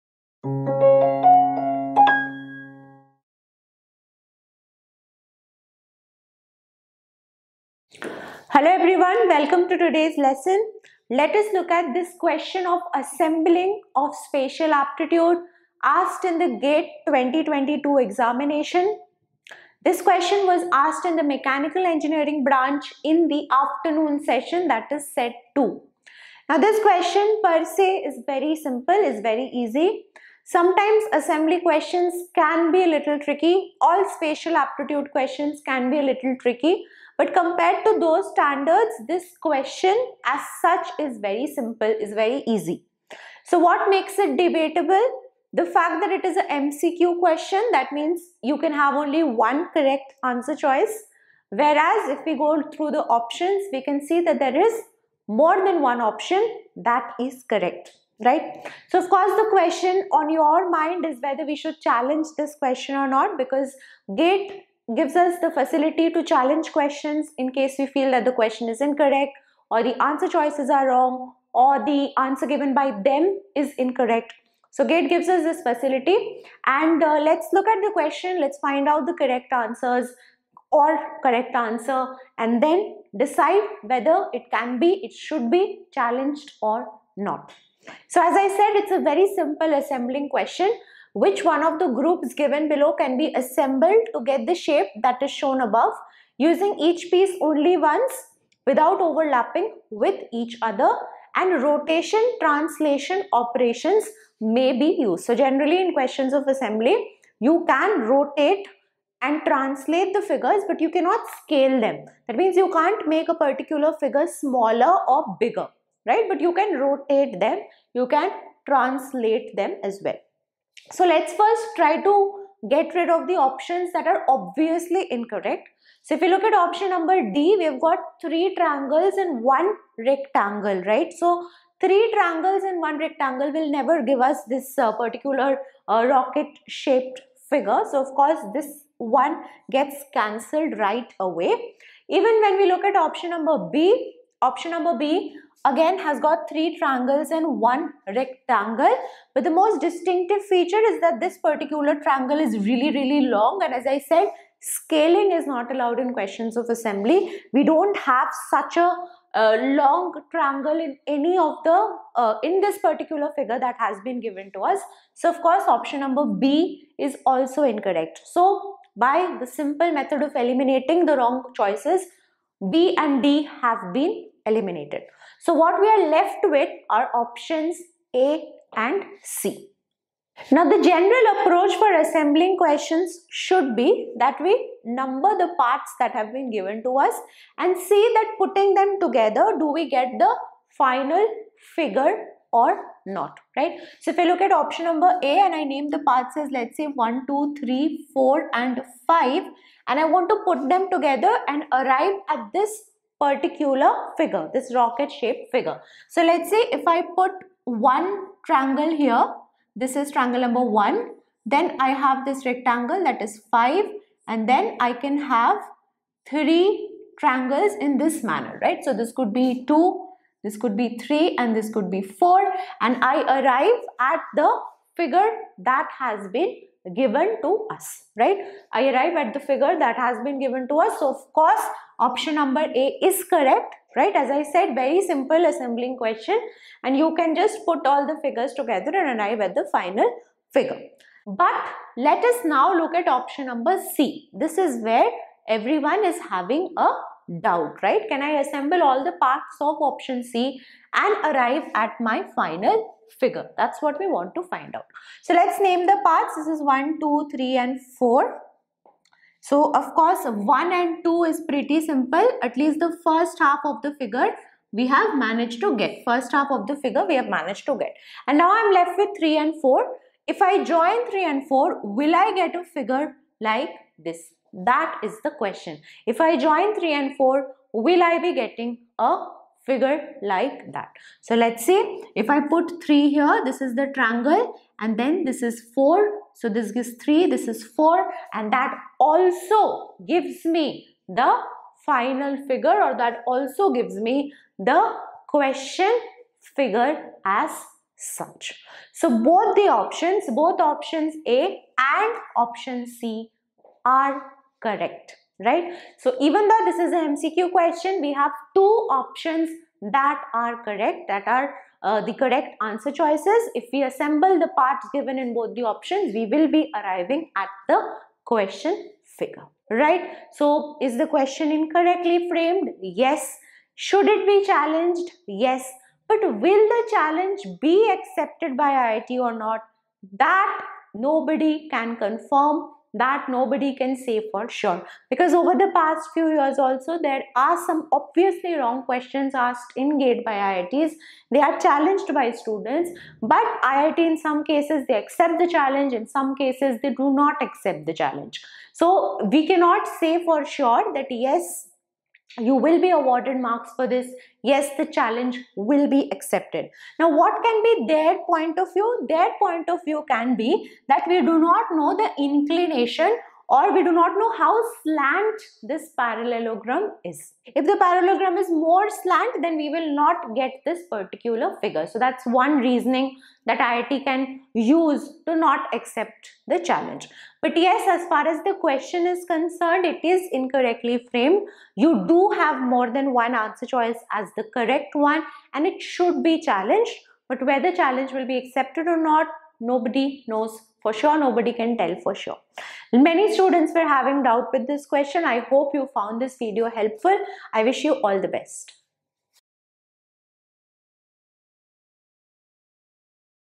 Should you challenge this Question asked in GATE 2022? | Spatial Aptitude - Assembling Question
Do you want to improve your numerical solving skills ?